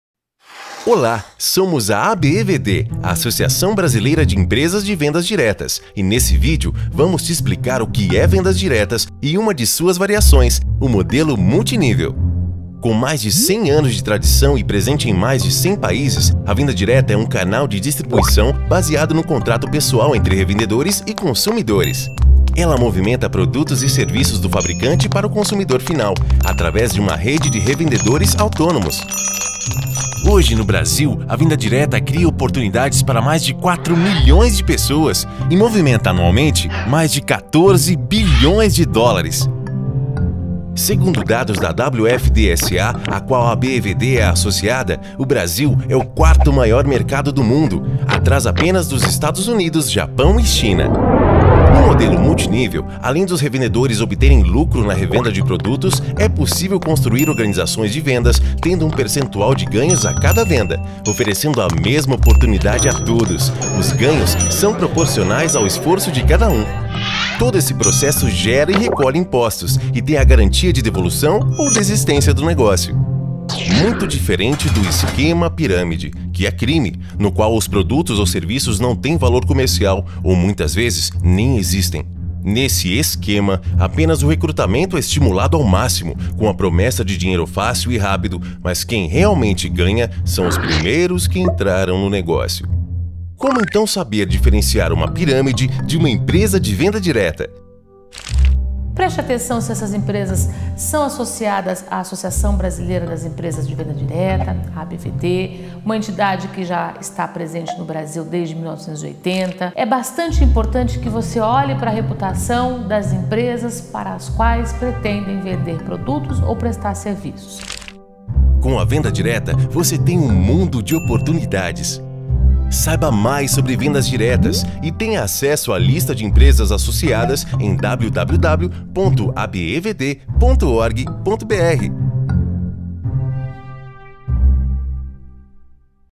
Marketing Multinível X Piramide - ABEVD " Associação Brasileira de Empresas de Vendas Diretas "
Veja a diferença de piramide financeira e o VERDADEIRO Marketing Multinível!!!